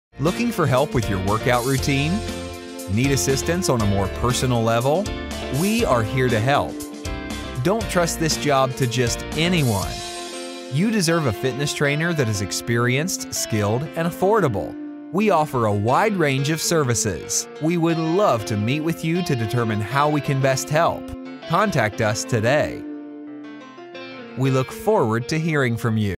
Fitness Training Video | 2D Cartoon Animation | Fitness Trainer
Video@click created this Fitness Training Video for a client's personal training services for the home. We chose 2D cartoon animation to depict the idea based on a problem and solution scenario. The target audience was the people looking for a home trainer in their area.

Need Fitness Training Video in a very short time and at an affordable price?